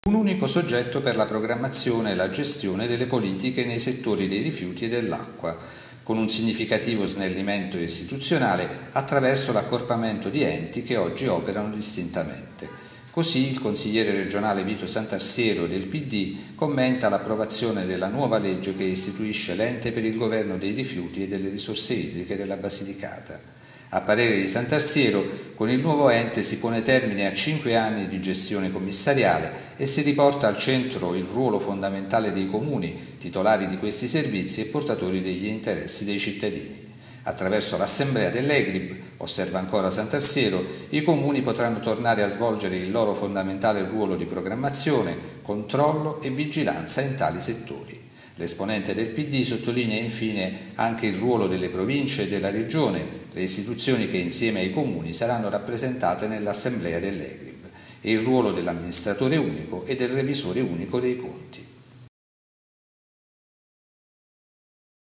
EGRIB, SANTARSIERO: UNICO SOGGETTO PER ACQUA E RIFIUTI
“Con il nuovo ente significativo snellimento istituzionale”